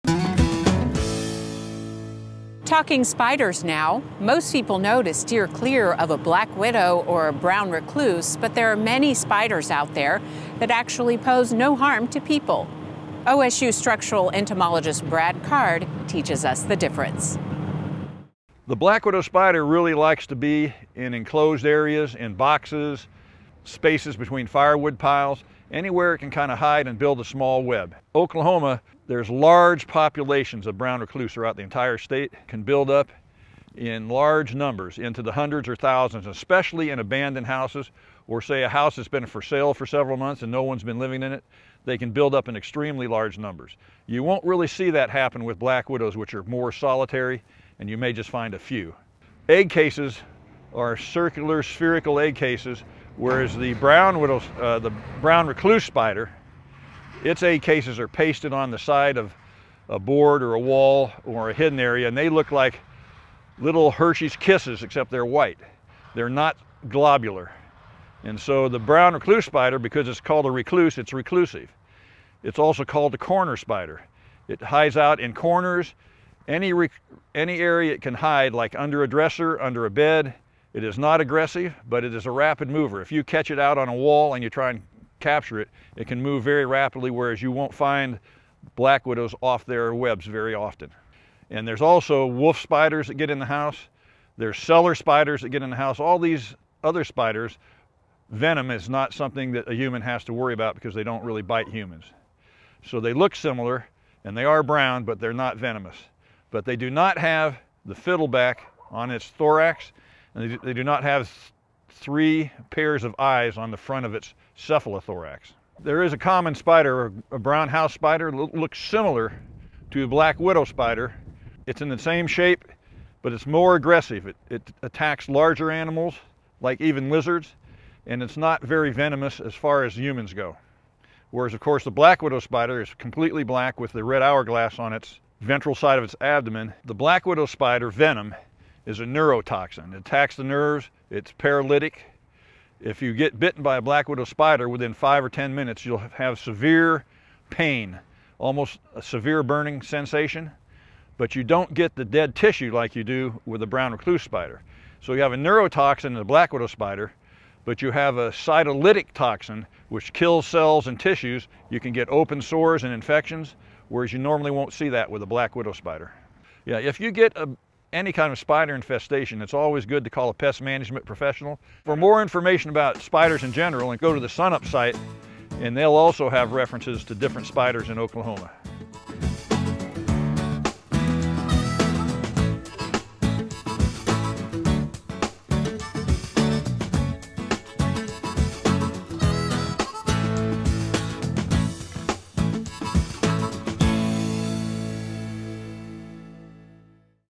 Identifying Spiders (3/7/20)
Brad Kard explains the facts about black widow and brown recluse spiders and helps identify the differences between them.